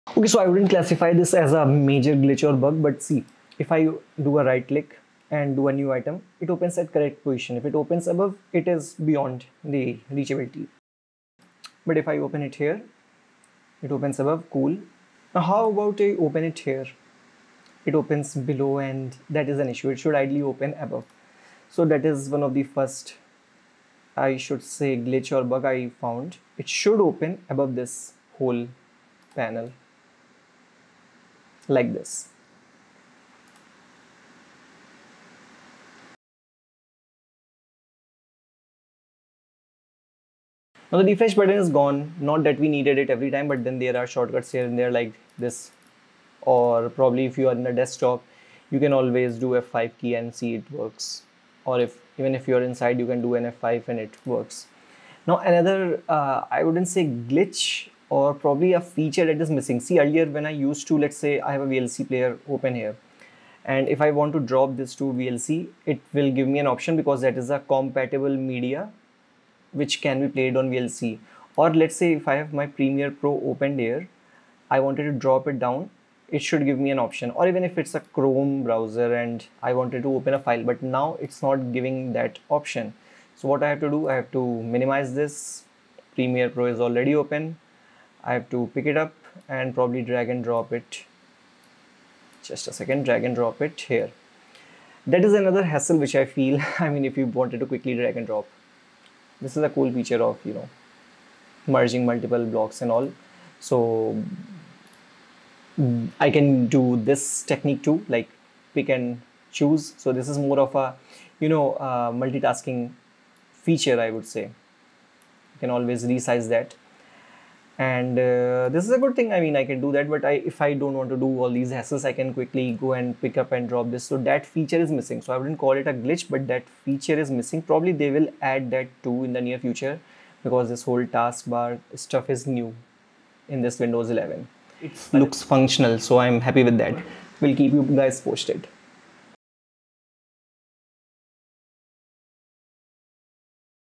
Windows 11 dev version 2 bugs/missing features | Premiere Pro and OBS Tested
This is just to showcase that overall OS is stable and old installed software is working great. Otherwise, everything is working great in fact just edited this video, and no issues so far. OBS (screen recording software ) and premiere Pro working great. It's just a few UI glitches here and there and missing features that used to exist.